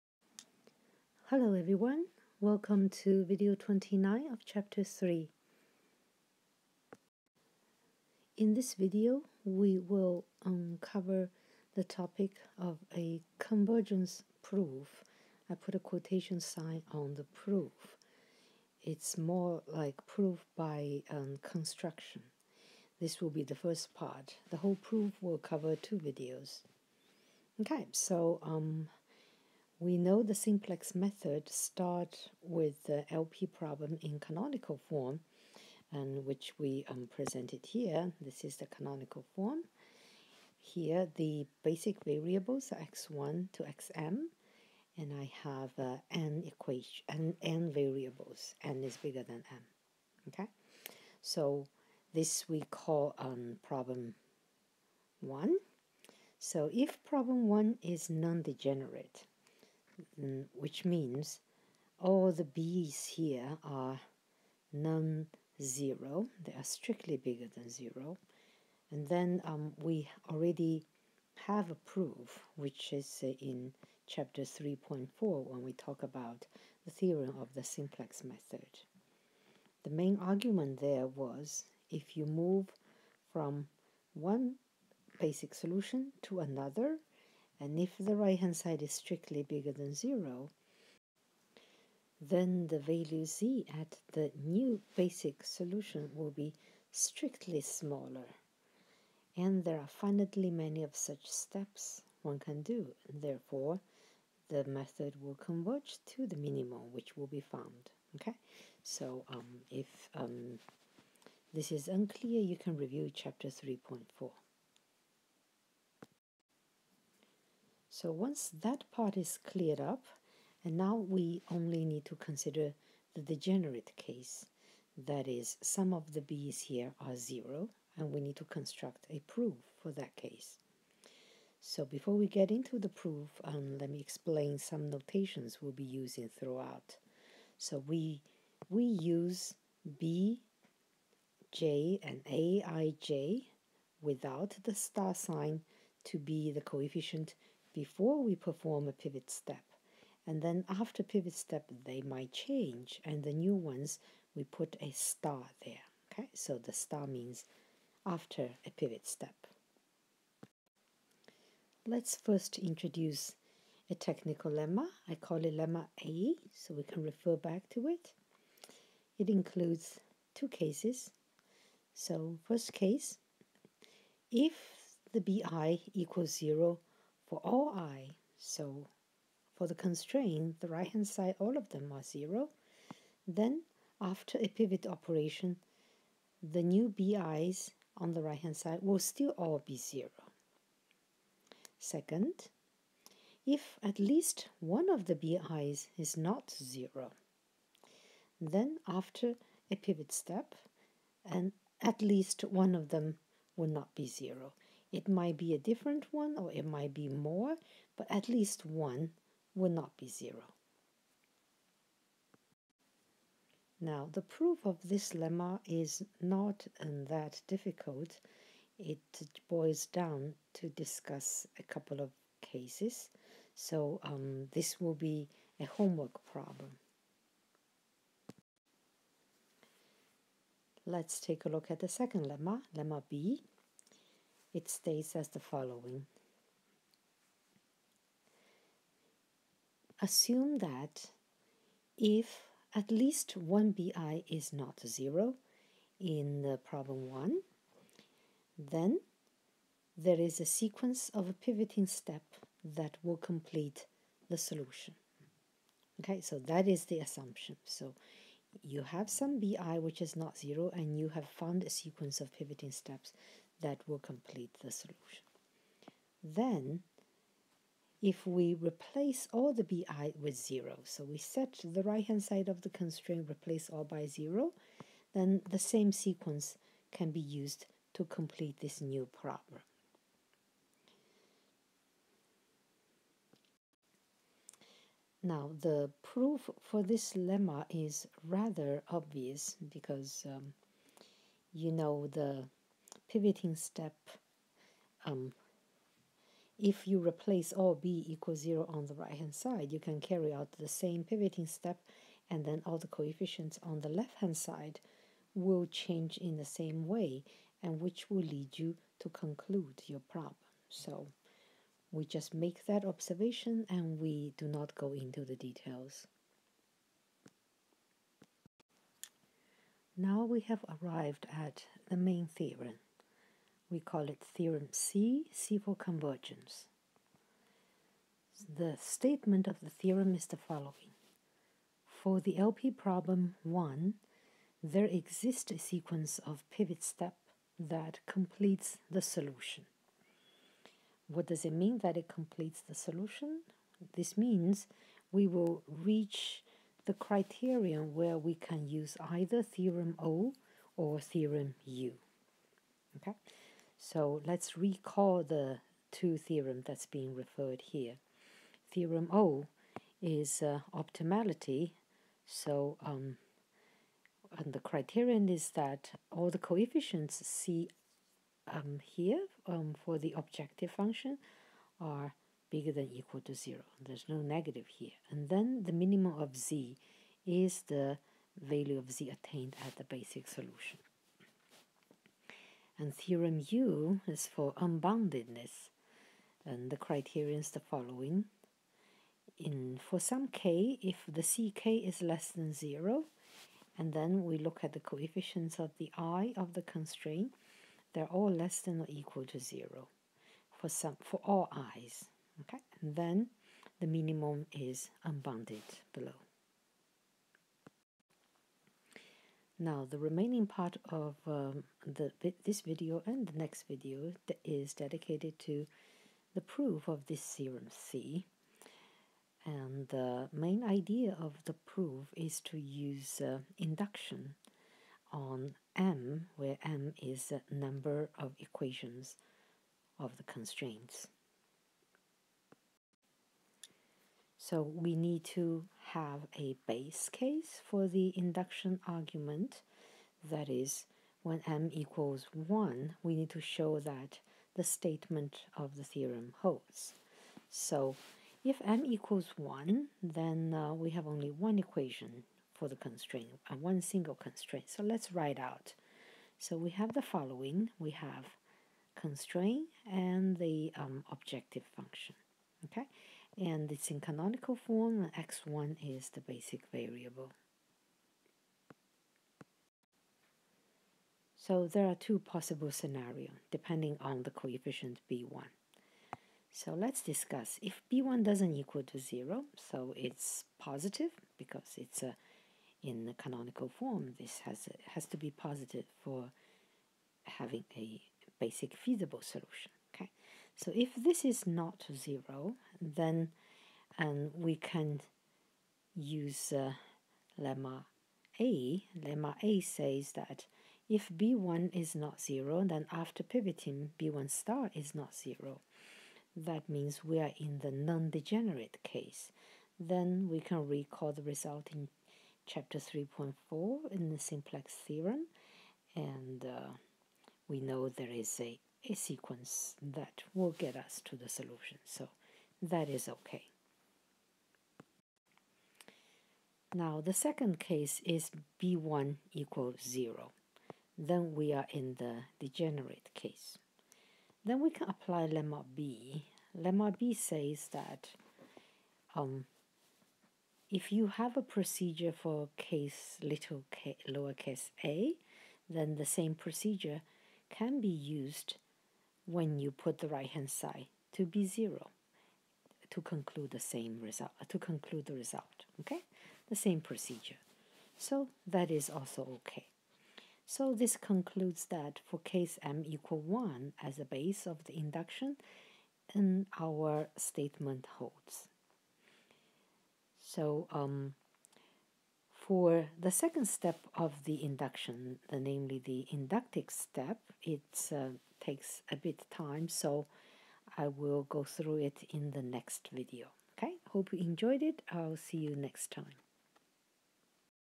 V3-29. Linear Programming. Convergence proof for Simplex method.
Math 484: Linear Programming. Convergence proof for Simplex method.
Wen Shen, 2020, Penn State University